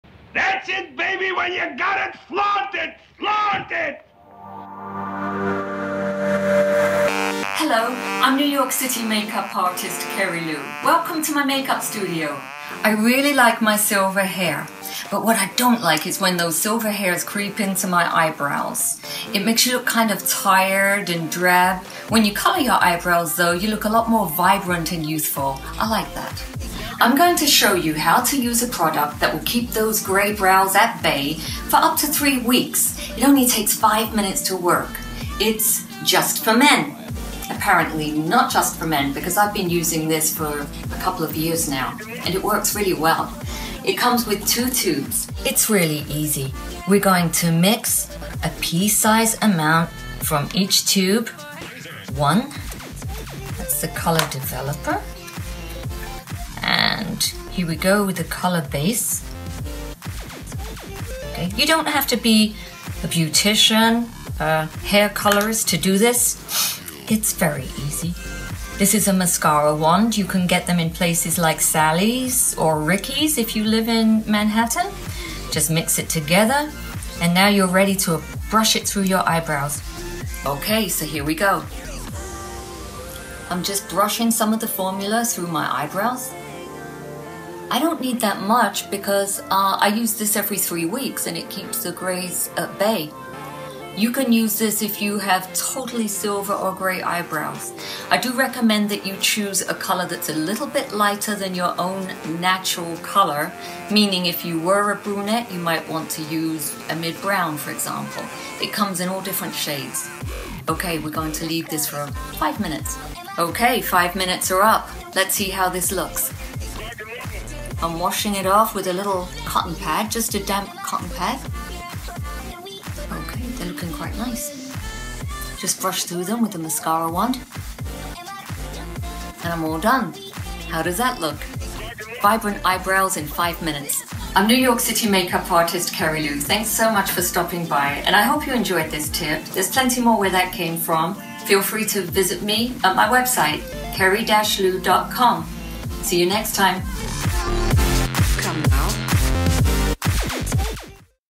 Quick Eyebrow Color - Tint Grey Hairs Using "Just for Men"
In this video, I demonstrate the quick and simple way that I use to tint my grey eyebrows.  I am using Just for Men Mustache and Beard formula.  I have been using this for years, and I am happy with the results. There is a warning on the box not to use on eyebrows, please read warnings, and how to apply.  I only use the tiniest application on the mascara wand, so there's nothing to drip.  Don't do this if you feel uncomfortable with it, or if you have sensitive skin.

Just for Men I use Blonde, which comes out kind of ashy.
Mascara wands